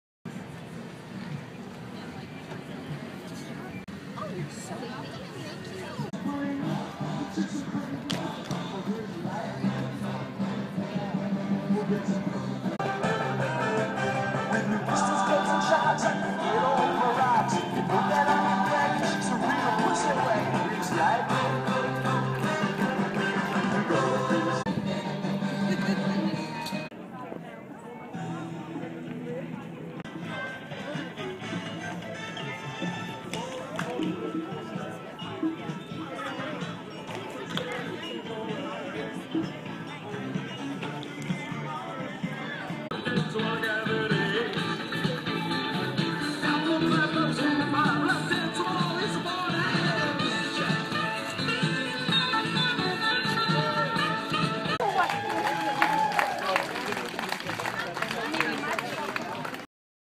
Art night out in Lafayette Colorado kicks off with flash mob by Louisville dance center.
Art night out in Lafayette Colorado kicks off with flash mob by Louisville dance center. @atsLafayette

By: C. Alan Crandall
Published on: June 20, 2014